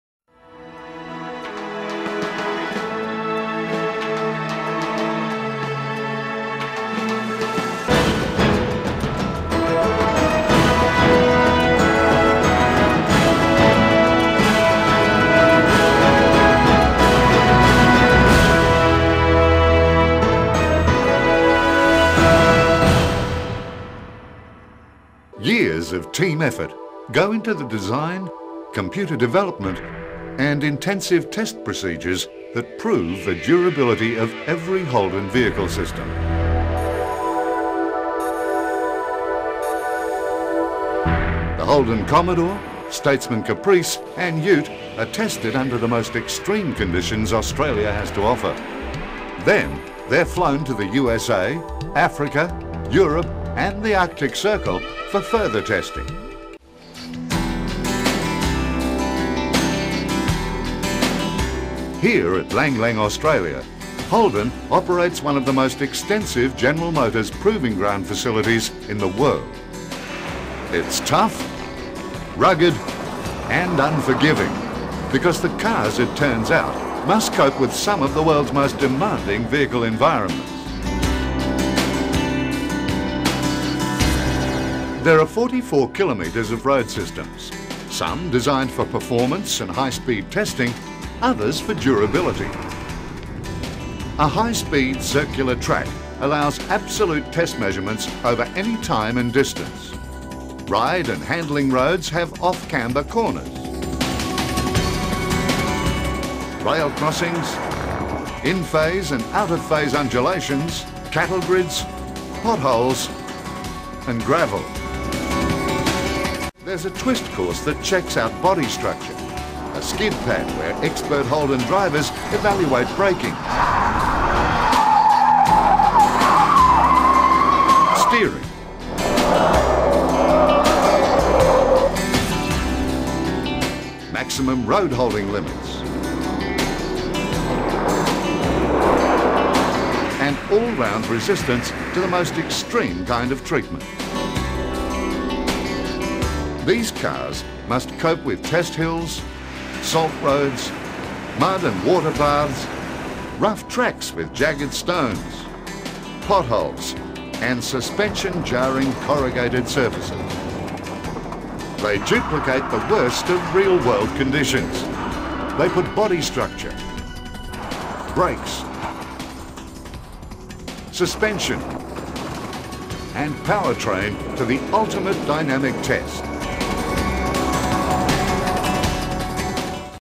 Holden vehicle development
a run through of holdens development at lang lang